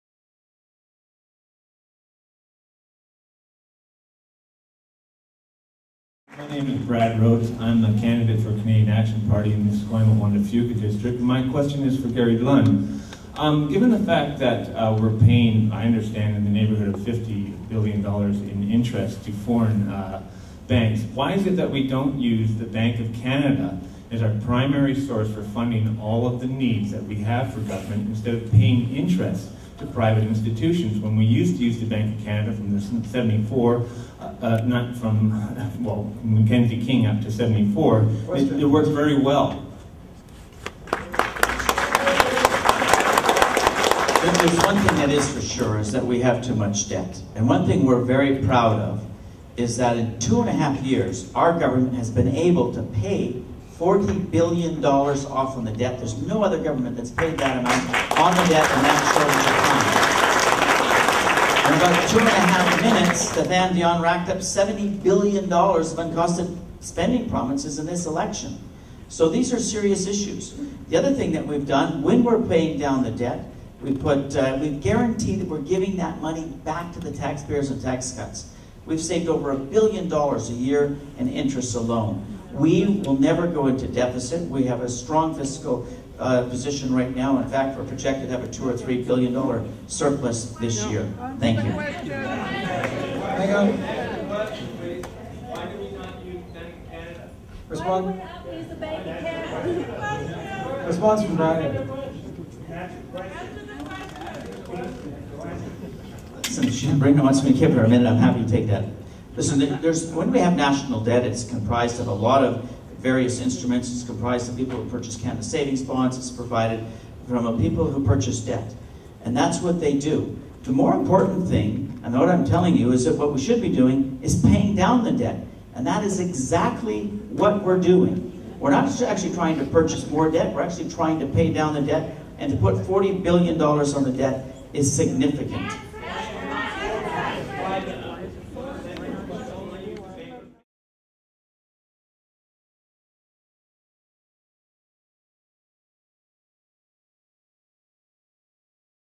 Answer the Question - Gary Lunn!
Brad Rhodes of the Canadian Action Party (2008) delivers a simple question to the Gary Lunn (Conservative) regarding the lack of use of the Bank of Canada over the past 30 years. It is common knowledge what the Bank of Canada was created for, it's just most mainstream puppets avoid the question like the plaque.

Unfortunately, Gary is given two chances to answer this simple question - and fails miserably both times.

Maybe he doesn't know the answer? Maybe he didn't hear the question correctly?

Either way its the same common silence that is becoming more evident as the political and global game unfolds.